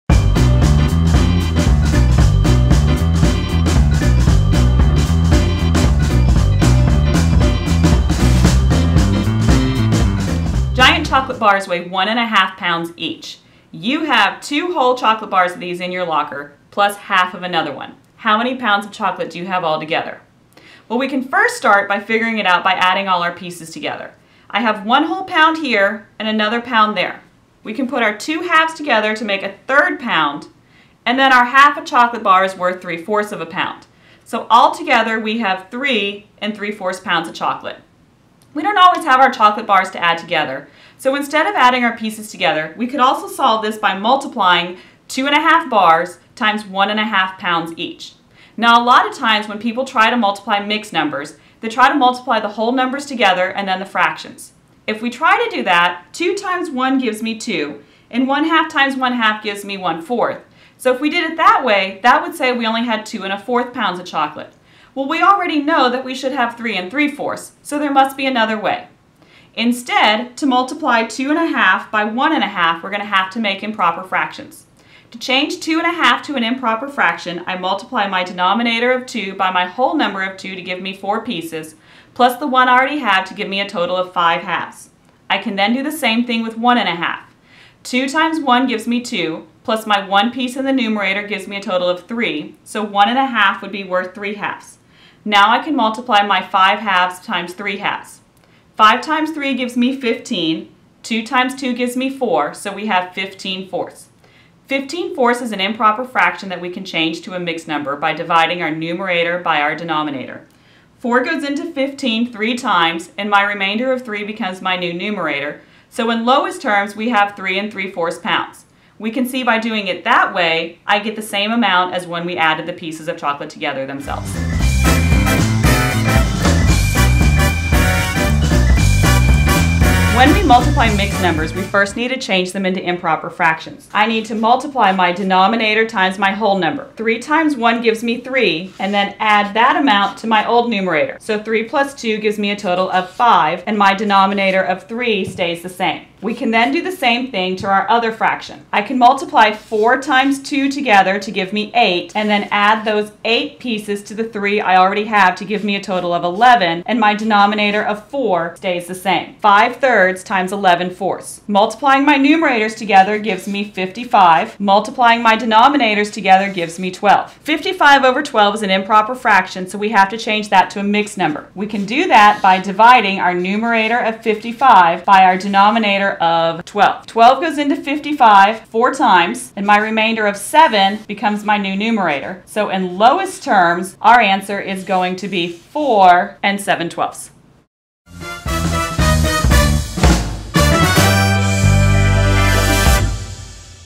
Multiplying Fractions: Mixed Number by Mixed Number
Learn how to set up and multiply with mixed numbers.